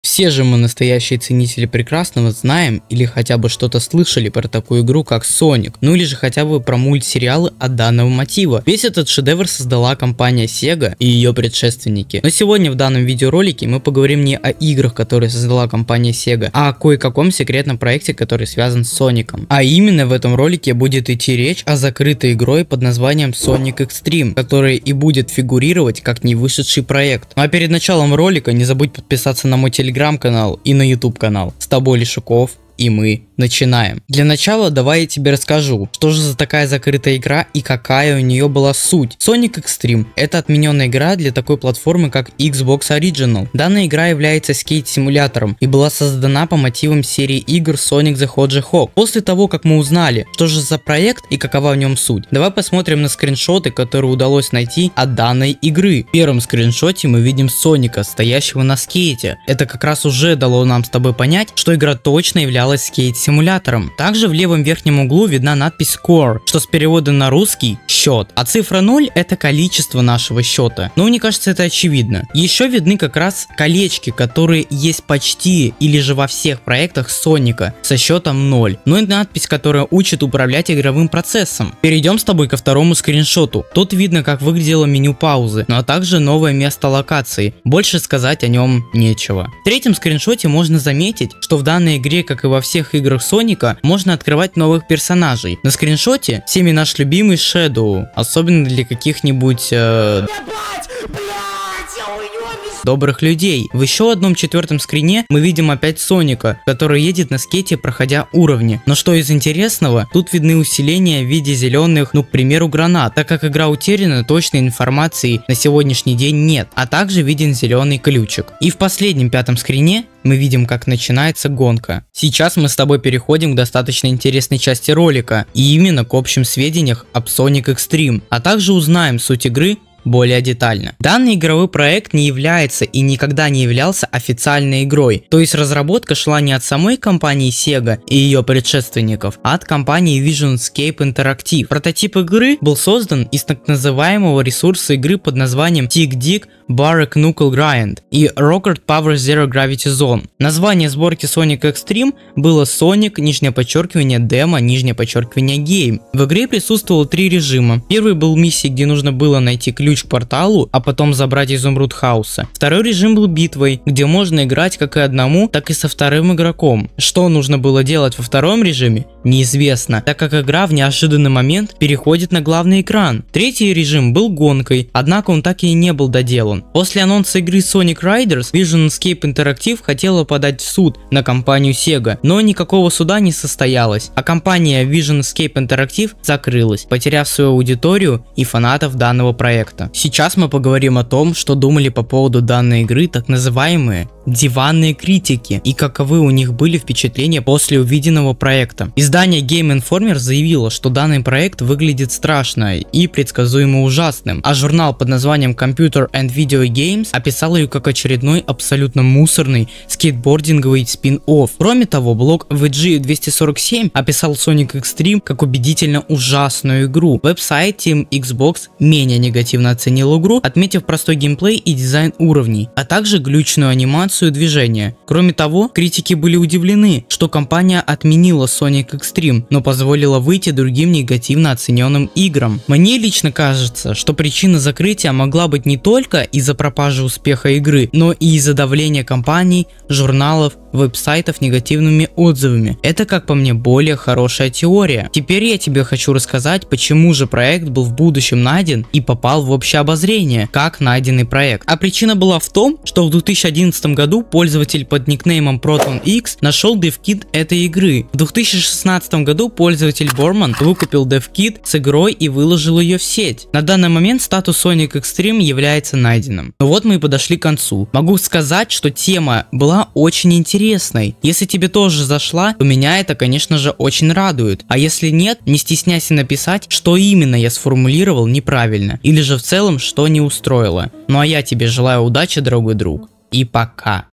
НЕ ВЫШЕДШАЯ ИГРА С СОНИКОМ! | Sonic Extreme, Lost media
Привет! с тобой как всегда leshukov(●'◡'●)!

Тайм коды:
0:00 - Вступление
0:35 - О закрытой игре
0:48 - Скрины из игры
2:03 - Общие сведения об игре
2:56 - Суд который не состоялся(Закрытие игры)
3:09 - Мнение экспертов по поводу Sonic Extreme
3:52 - Мнение Leshukov о причинах закрытии игры
4:05 - Почему проект раскрылся и был найден?
4:27 - Конец